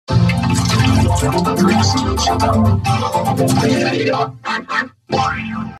{FRIDAY THE 13TH 2021 SPECIAL} An Audio Effect I'll Call "FridayThe13thFlangedSawChorded"
(A. K. A. : Klasky Csupo Robot Logo With FridayThe13thFlangedSawChorded)

Happy Friday The 13th 2021 Everyone!